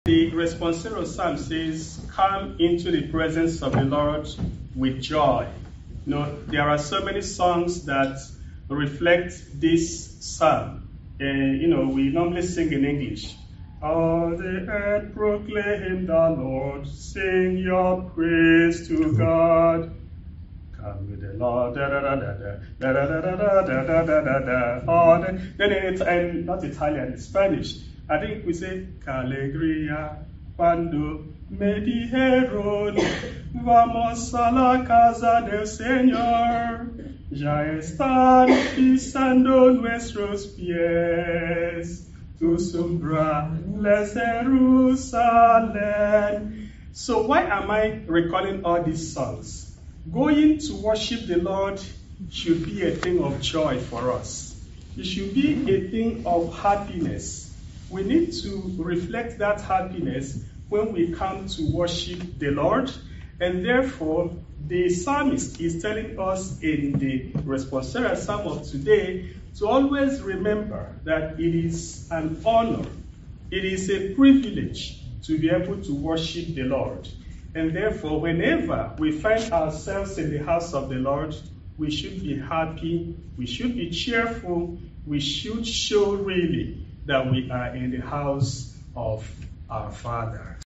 Ekpendu ACC homily podcast 9-6-19: Worship and serve the Lord with joy.
Homily of Fr. Chidi Ekpendu at Archdiocese of Los Angeles Catholic Center (ACC) noon Mass 6 Sept. 2019: It's a privilege to worship and serve God.